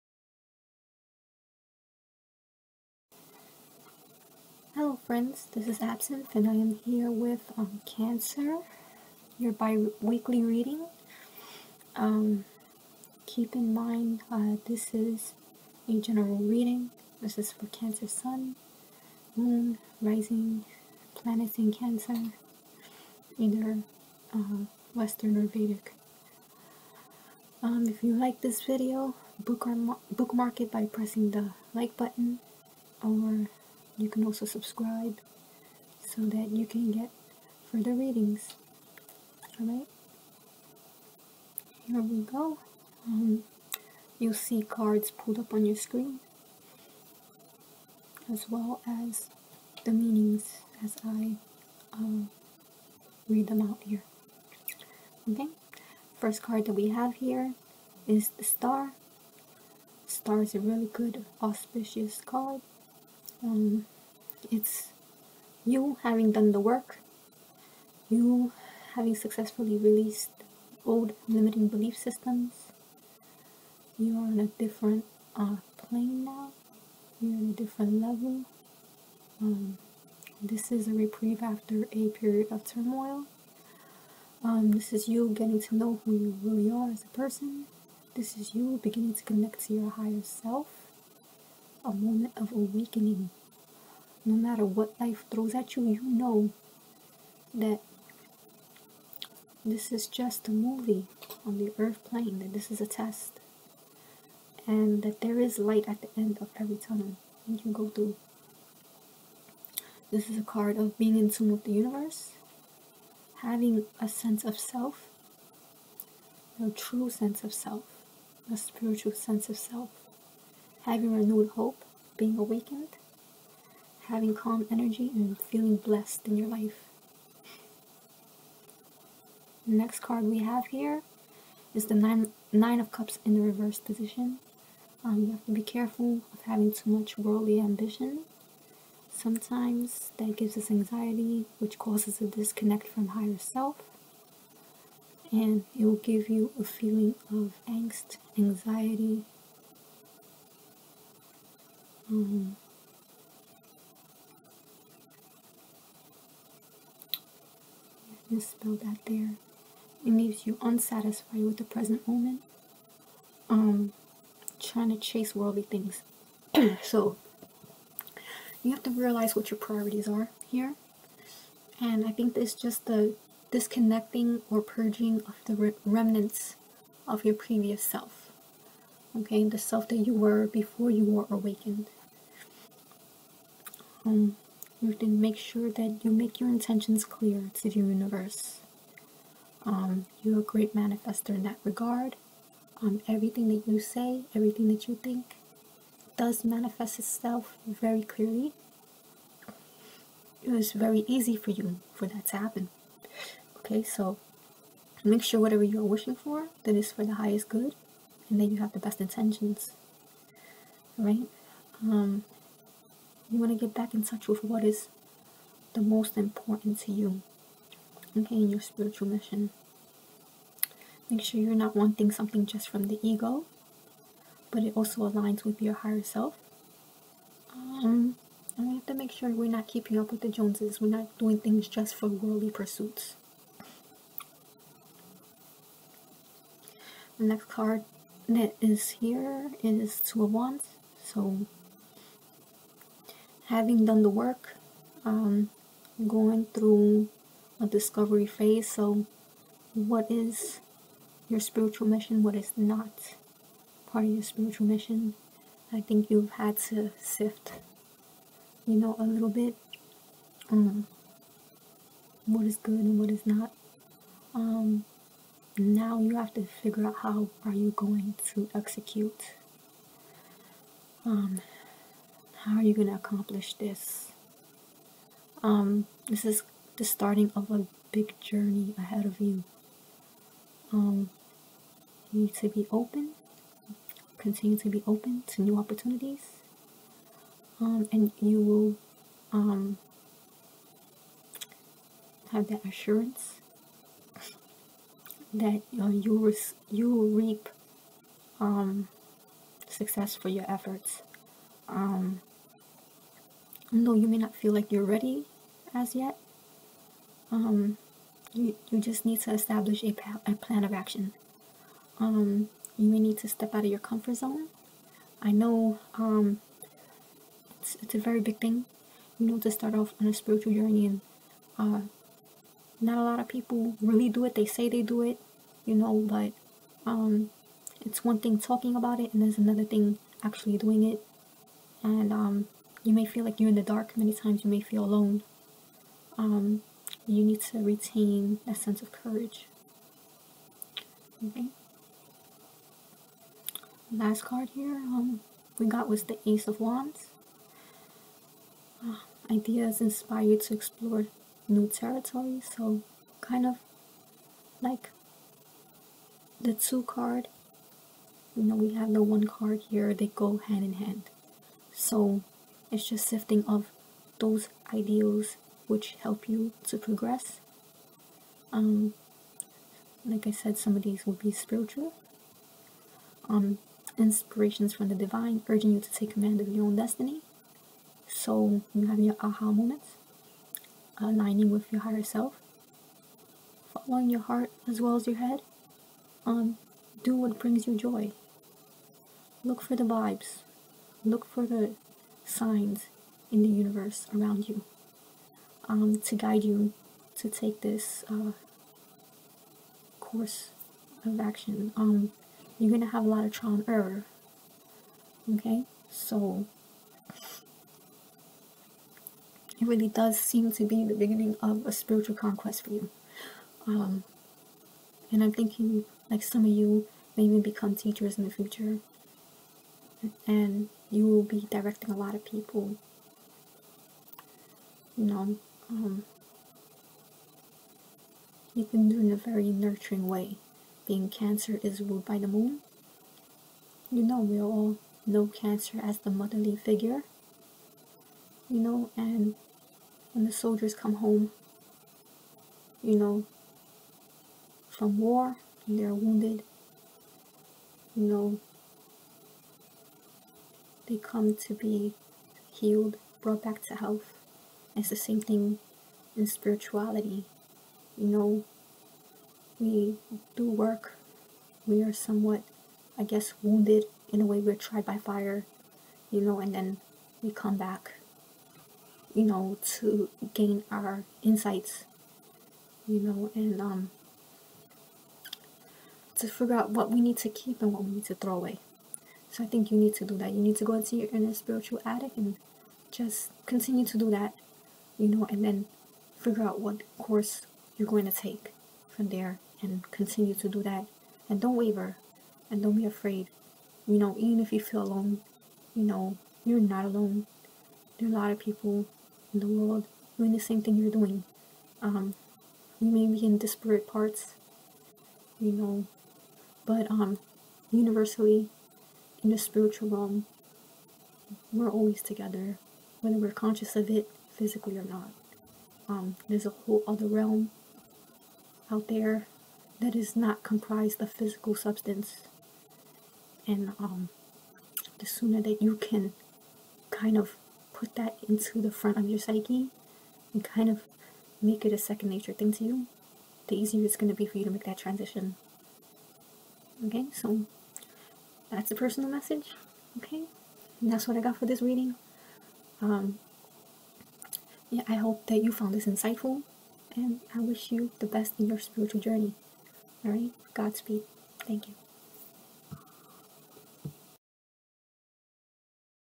Cancer Biweekly Tarot Reading, Call to a Spiritual Adventure, February 2021
This is a general tarot card reading. This may or may not resonate with you, if not there will be others. Look at the positions in your birth chart to see what is most prominent.
I do not do personal readings at this time.
Deck I am using is the Steampunk tarot by Barbara Moore